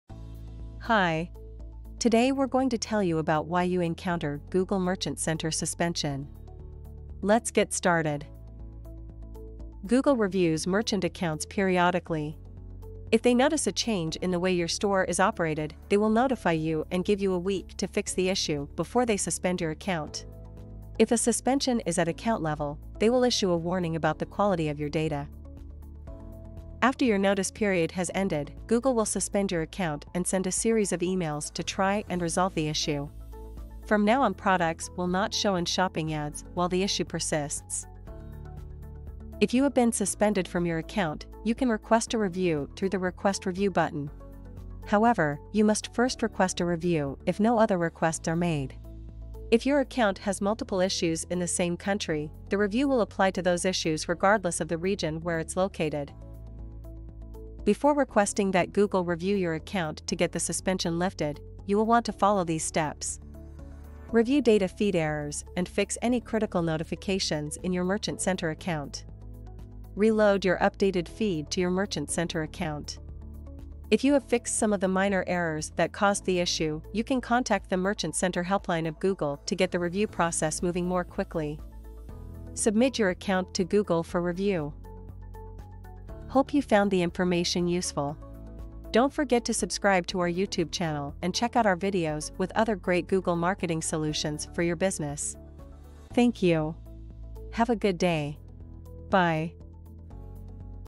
Account Level Google Shopping Suspension and How to Fix It
Would you like to fix Google Ads account suspension?

Hi! Today we’re going to tell you about Why You Encounter Google Merchant Center Suspension.

Google reviews merchant accounts periodically. If they notice a change in the way your store is operated, they will notify you and give you a week to fix the issue before they suspend your account. If a suspension is at account level, they will issue a warning about the quality of your data.

After your notice period has ended, Google will suspend your account and send a series of emails to try and resolve the issue. From now on products will not show in Shopping ads while the issue persists.

Google Shopping Account-Level Review Process

If you have been suspended from your account, you can request a review through the Request review button. However, you must first request a review if no other requests are made. If your account has multiple issues in the same country, the review will apply to those issues regardless of the region where it's located.

Before requesting that Google review your account to get the suspension lifted, you will want to follow these steps:

Review data feed errors and fix any critical notifications in your Merchant Center account.
Reload your updated feed to your Merchant Center account.
If you have fixed some of the minor errors that caused the issue, you can contact the Merchant Center helpline of Google to get the review process moving more quickly.
Submit your account to Google for review.

00:00 - 00:08 - Welcome to our video!
00:09 - 00:25 - Google reviews merchant accounts
00:26 - 00:38 - End of notice period
00:39 - 00:56 - Requesting Google review
00:57 - 01:24 - Conclusion
01:25 - 01:40 - In case you want further help

USEFULL RESOURSES